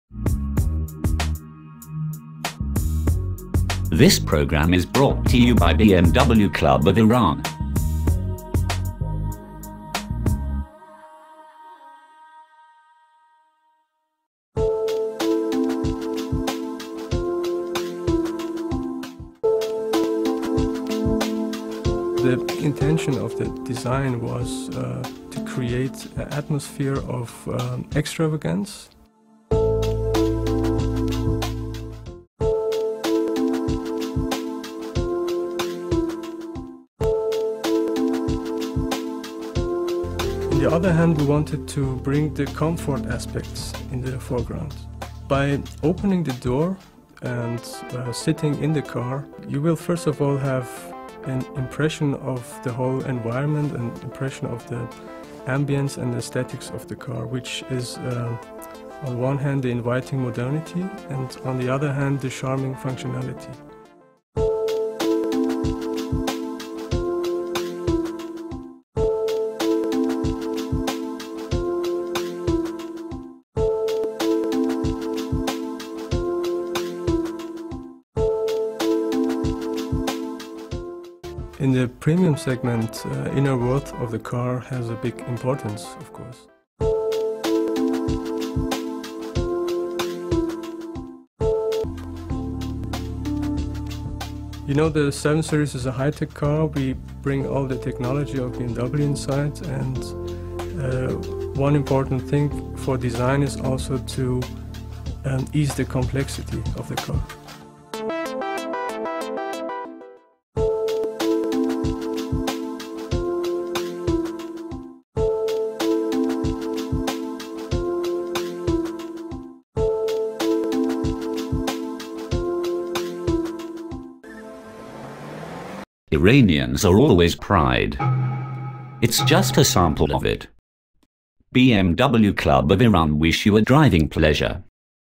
BMW 7 Series- Interior
Designing the New BMW 7 Series offers a unique behind the scenes look at the BMW design. Nader Faghihzadeh describes the challenges and highlights experieced while designing the ambience for the new BMW 7 Series' interior.